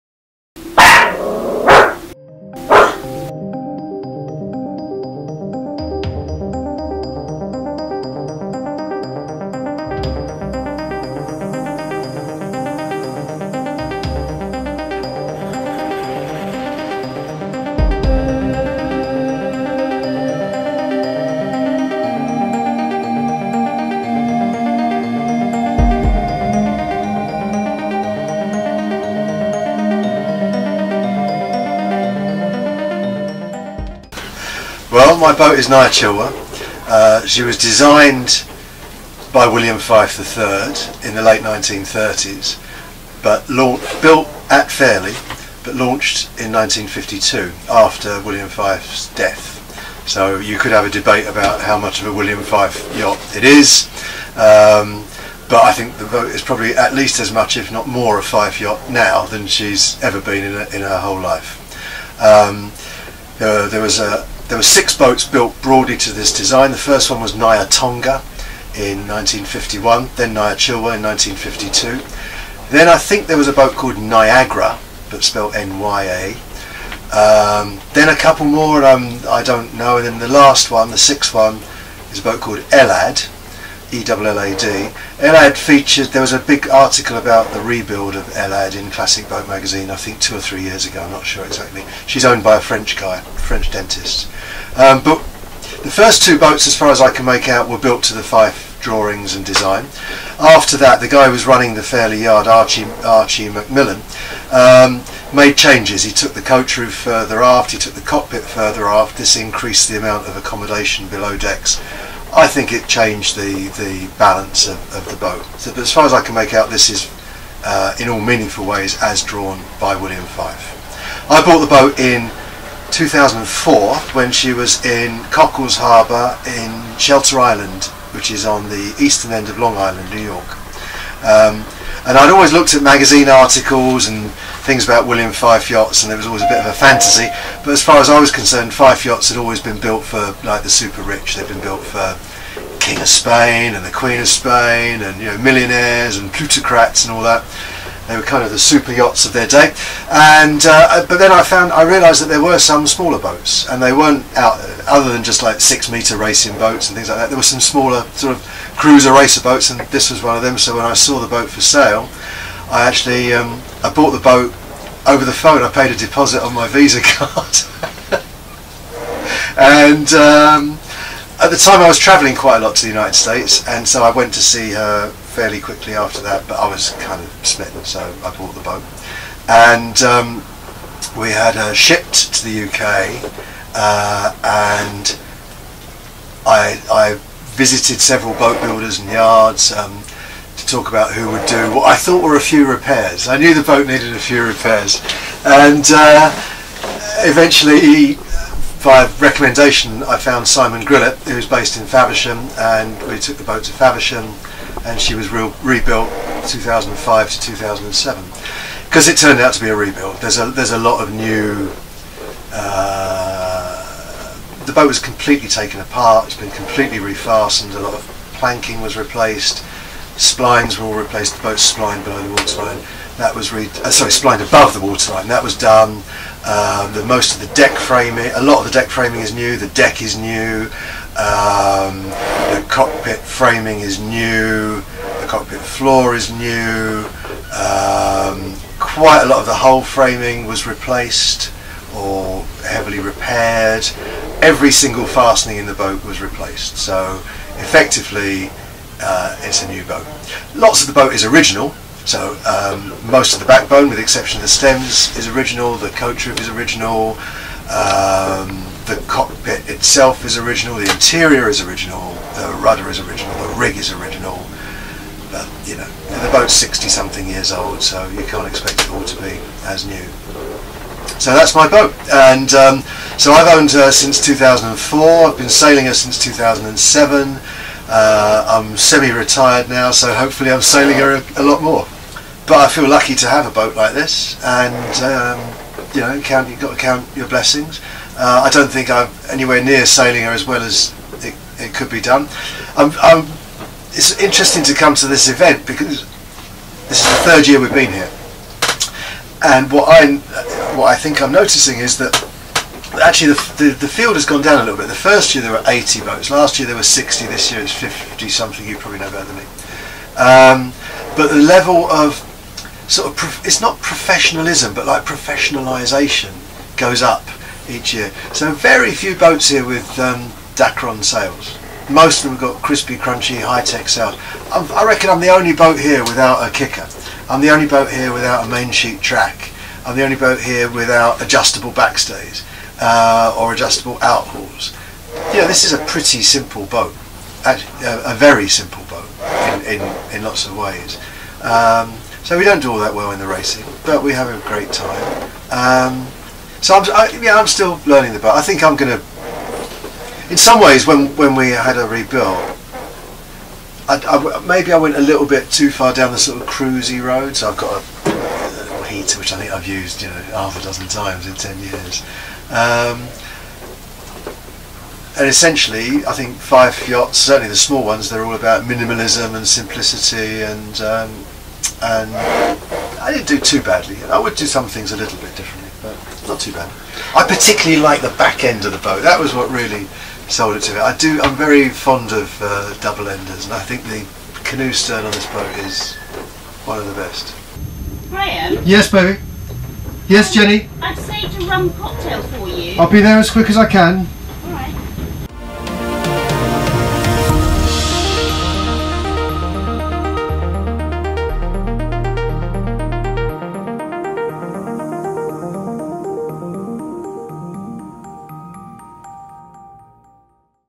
My Classic Boat. Fife Sloop 33ft 1952
I've had this one on sitting on the shelf for a bit, but thought it was time to bring it out and brighten up a cold snow bound February for you. A beautiful classic yacht filmed at the Panerai Classic week in Cowes, plus an interesting on board interview with owner Graham Dallas.